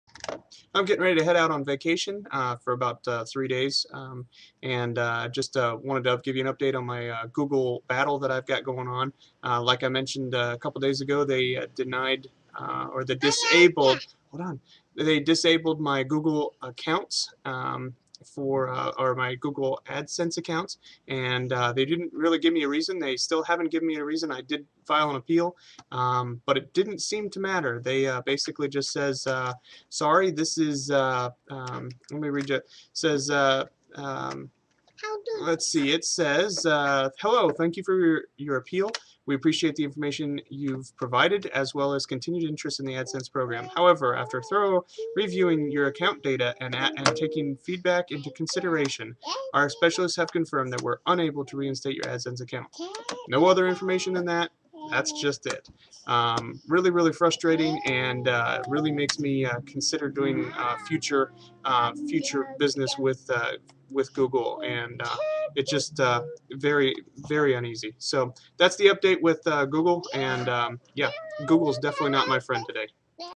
2011.05.13 - My continued battle with Google
I briefly talk about battle with Google and how they disabled my adsense account with no explanation.  I don't even know if I did something wrong.  They simply said "Hello,

After reviewing our records, we've determined that your AdSense account
poses a risk of generating invalid activity. Because we have a
responsibility to protect our AdWords advertisers from inflated costs due
to invalid activity, we've found it necessary to disable your AdSense
account. Your outstanding balance and Google's share of the revenue will
both be fully refunded back to the affected advertisers.

Please understand that we need to take such steps to maintain the
effectiveness of Google's advertising system, particularly the
advertiser-publisher relationship. We understand the inconvenience that
this may cause you, and we thank you in advance for your understanding and
cooperation.

If you have any questions or concerns about the actions we've taken, how
you can appeal this decision, or invalid activity in general, you can find

Sincerely,

The Google AdSense Team"

What a crock!